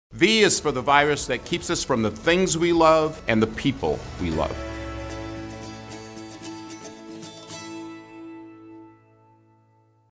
V is for… featuring ProMedica’s Randy Oostra
V is for the vaccine that will save thousands of lives and bring us back together again.